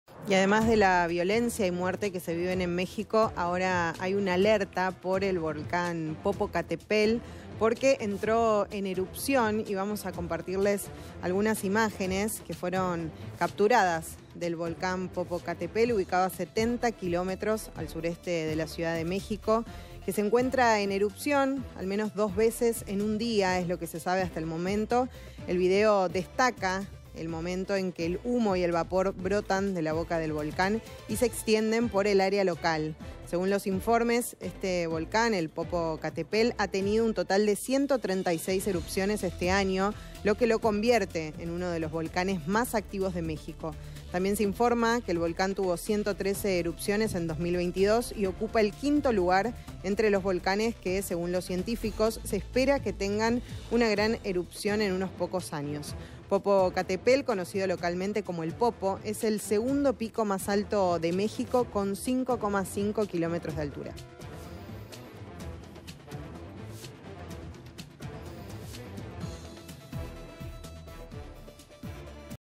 MÉXICO | Alerta por el volcán Popocatépl
👉🏻Las imágenes grabadas capturaron el volcán Popocatépl, ubicado a 70 kilómetros al sureste de la Ciudad de México, en erupción dos veces en un día.

👉🏻El video destaca el momento en que el humo y el vapor brotan de la boca del volcán y se extienden por el área local.
Según los informes, el volcán Popocatépetl ha tenido un total de 136 erupciones este año, lo que lo convierte en uno de los volcanes más activos de México.